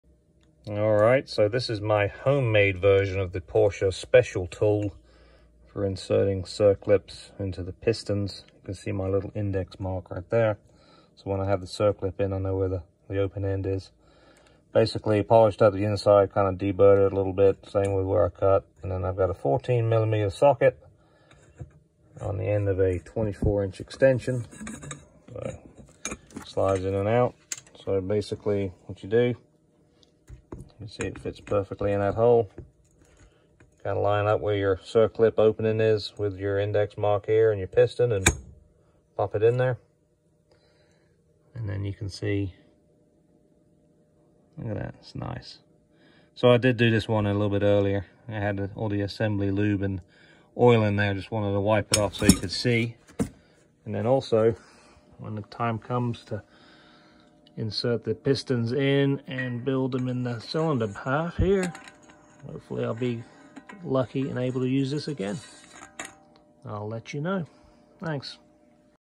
Home built Porsche wrist pin circlip installation tool. Rebuilding my m96. 996 Carrera 4S 4 liter
This is clearly no substitute for the Porsche tool but maybe it will help someone out there save a bit of hard earned money. One thing to note, since my JE locks didn't have the little hook on them, you want to orient the gap in the circlip away from the notch in the lock hole. Will make it much easier to remove the circlips, should you ever need to down the road.

1” conduit and a 16mm socket on a long extension.

Works like a charm.